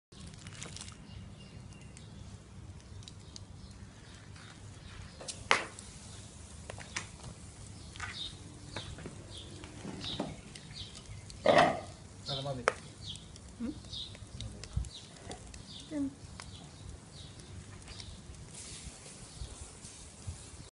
Grilling lamb steaks
Best idea ever, we only added Omani garlic and salt to the meat and it turned out very delicious !